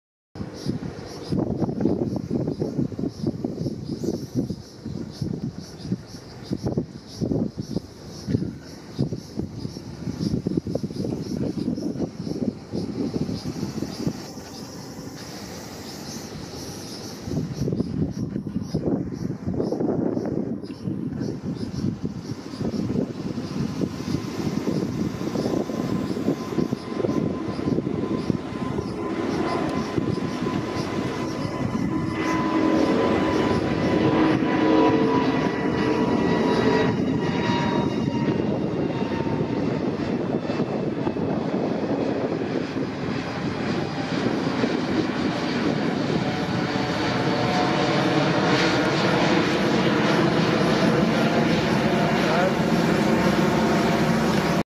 瀬長島より 那覇空港 JAL A350 4K離陸動画 2021.6.4
緊急事態宣言発令中　沖縄移住者の気分転換